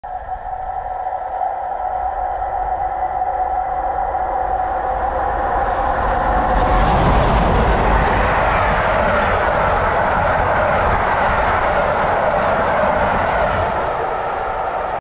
West Coast Railways Double Header Class 47 Heaven
West Coast Railway's 47 804 & 47 772 speeds past Eaton Lane Crossing 2/4/23 with ECS service 5Z55 Norwich to Carnforth